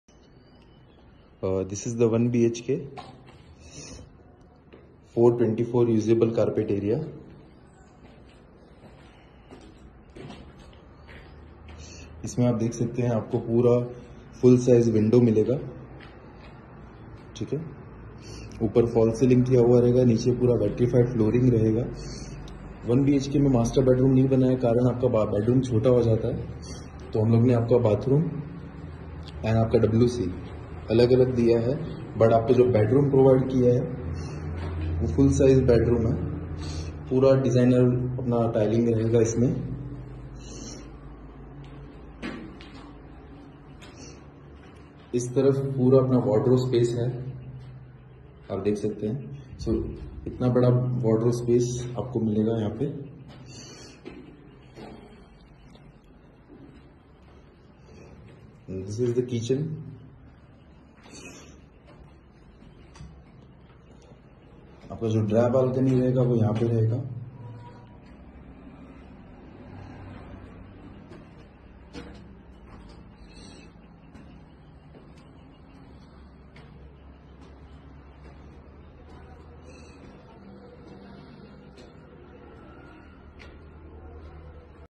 palghar west flat available contact:-9137284774
*Mahavir Complex*
*Vinayak Builders Palghar (W)*
_Flats Starting from 22 Lakhs_

• *1BHK:*
: 360 Useable Area
: 380 Useable Area
: 424 Useable Area

• *2BHK:*
: 473 Useable Area
: 603 Useable Area
: 628 Useable Area
: 636 Useable Area

*Only 2km from Palghar Station*

*External Amenities:*

• 3 different types of garden:-
• Ladies corner with gazibo
• Sufficient open parking space
• Cctv with security
• Grand entrance ( gated society)

*Internal Amenities:*
• Modular kitchen
• False ceiling
• Virtrified flooring
• High quality of sanitary fitting
• Texture wall.
• Geyser
• RO

Near By *(Beach / Resort Resturant)* 🌊
•Shirgoan Beach
•Mahim beach
•BhasKarwadi Agro Tourism Resort
•Jaldevi Agro Tourism Resort

Near By *(School's & college,s)* 🏫

•Sundaram central school
•Twinkle star English High school
•Jawahar Navodya vidyalaya central
•J.P internal national High school
•S.T John college of engineering and management

Near By *(Hospital's)* 🏥🚑
•Dhada Hospital
•Philila Hospital
•Ganesh Hospital
•M.L Dhavle Hospital
•Relief Hospital (Orthopaedic &Surgical)